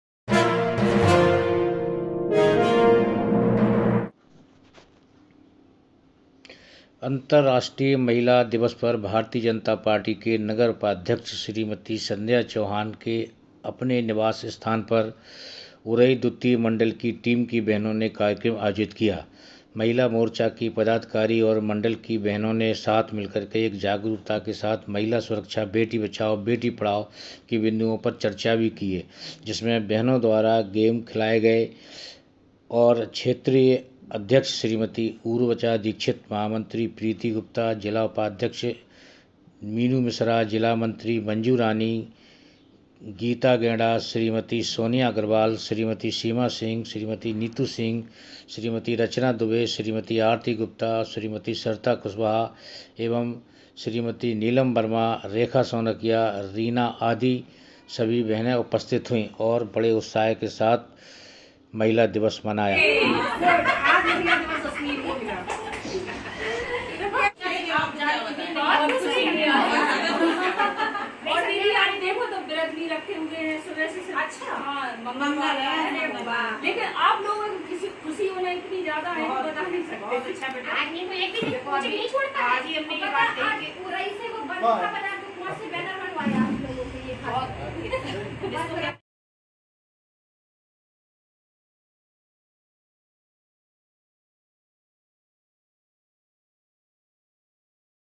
उरई की दुतीय महिला मंडल की टीम ने मनाया अन्तर राष्ट्रीय महिला दिवसः ।akhandbharattv
उरई की महिला मंडल टीम ने नगर उपाध्यक्ष संध्या चौहान के घर पर मनाया महिला दिवसः इस मौके पर  महिला मोर्चा की पदाधिकारी  सभी बहिनें मौजूद रही आपस मे एक दूसरे का स्वागत किया